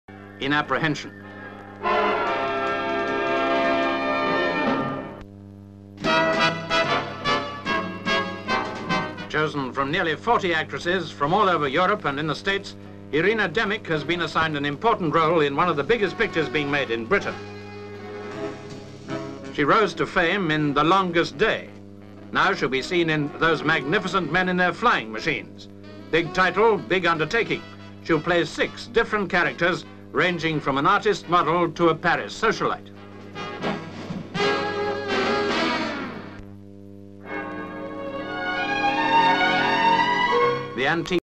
THE GIRL WITH "THOSE MAGNIFICENT MEN"
(15 Jun 1964) Chosen from nearly forty actresses, after tests all over Europe and the States, Irina Demick has been assigned an important role in one of the biggest pictures being made in Britain, "Those Magnificent Men In Their Flying Machines".